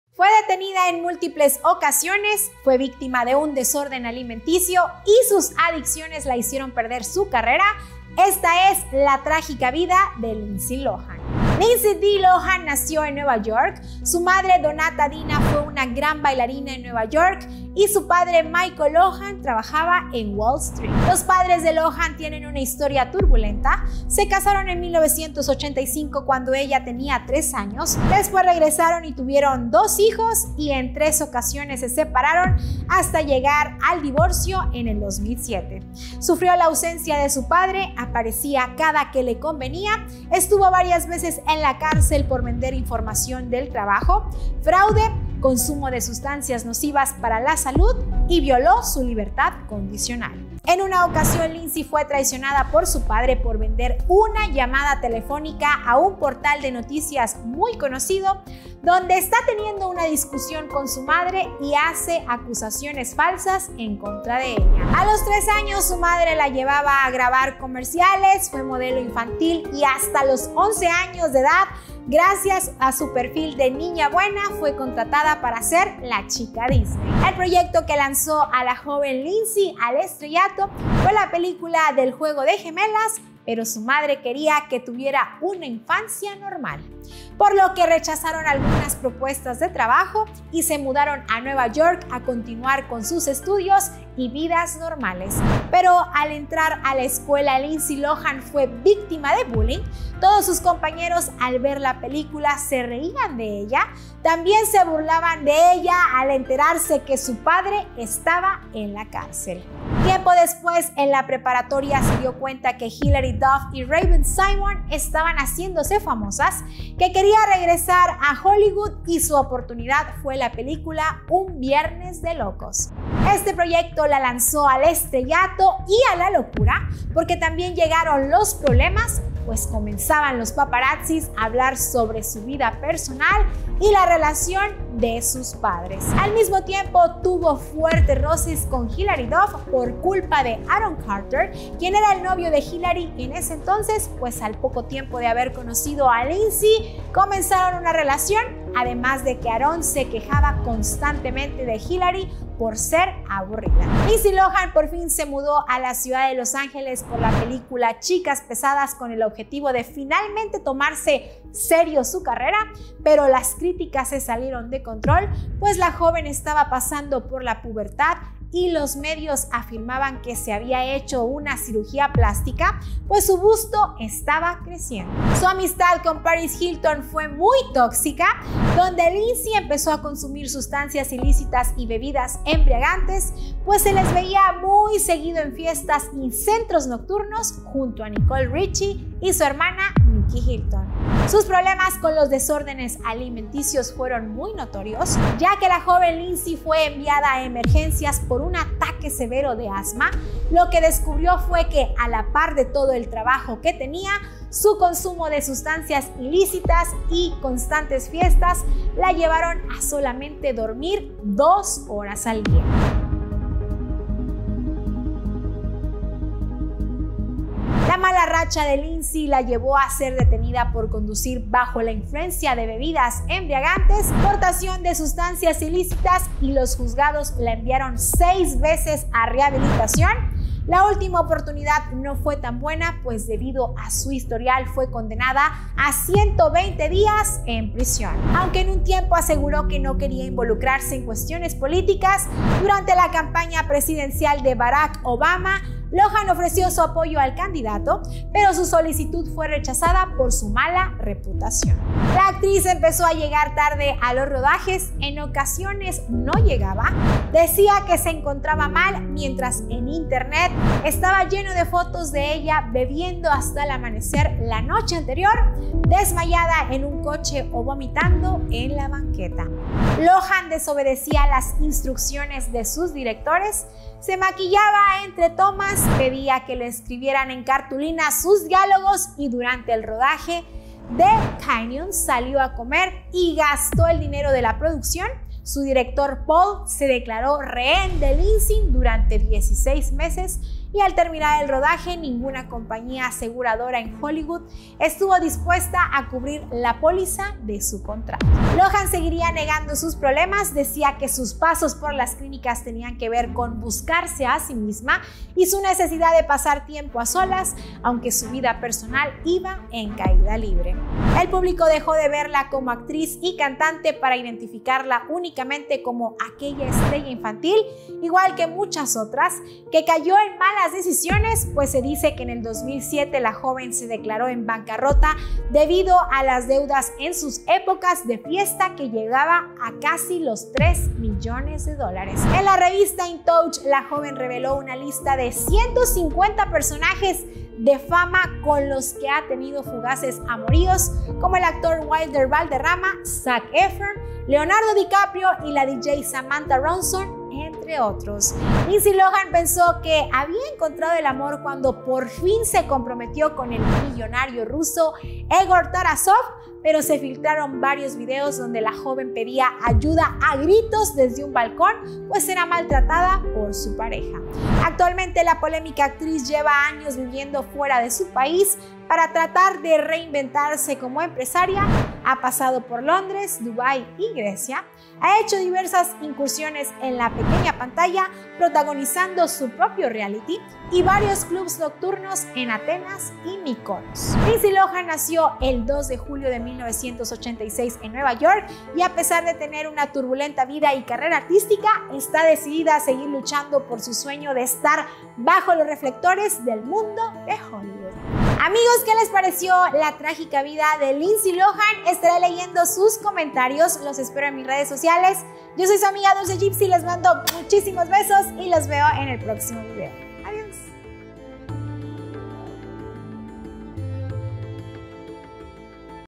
La triste vida y final de Lindsay Lohan
Fue detenida en múltiples ocasiones, fue víctima de un desorden alimenticio entre otras graves eventos. Dulce Gipsy nos revela los detalles de la vida de la estrella Lindsay Lohan en @DulceGipsy

BADABUN NETWORK ©